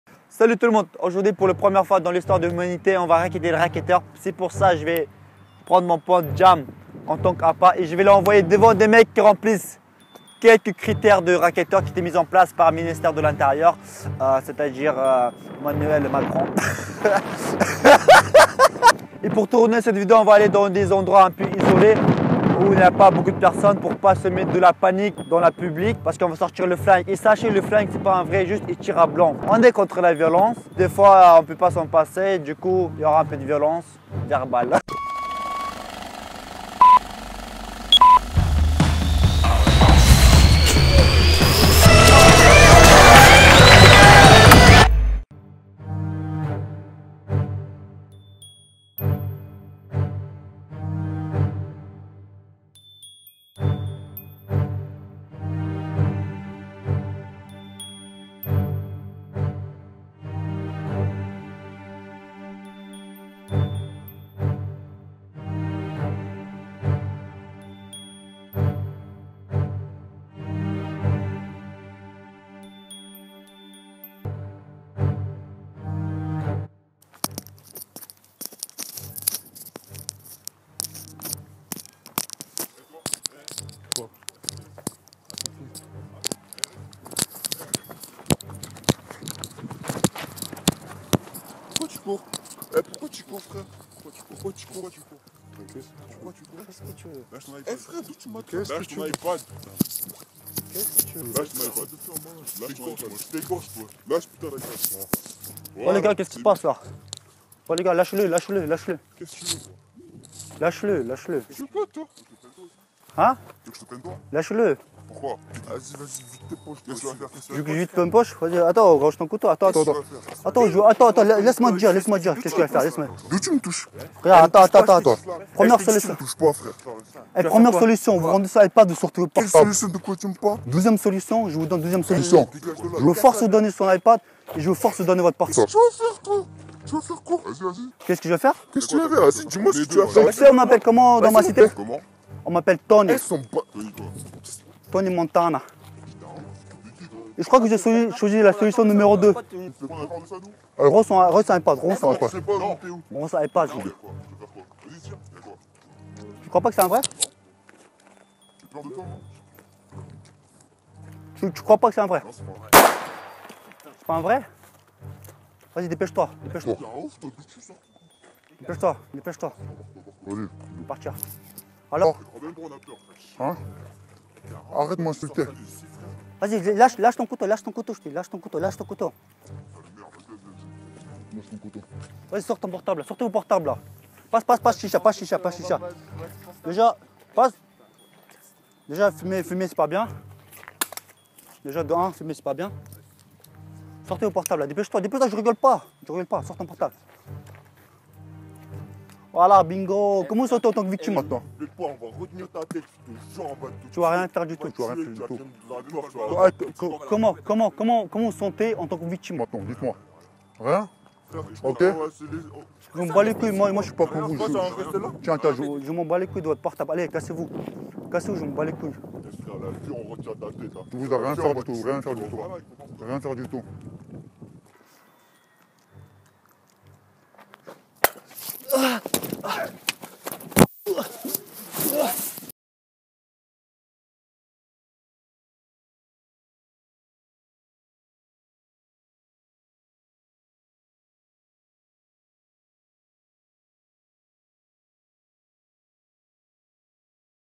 Racketter des Racketteurs | Robbing Robbers (real life)
dans cette vidéo on rackette des racketteurs

Artiste : __url_artiste__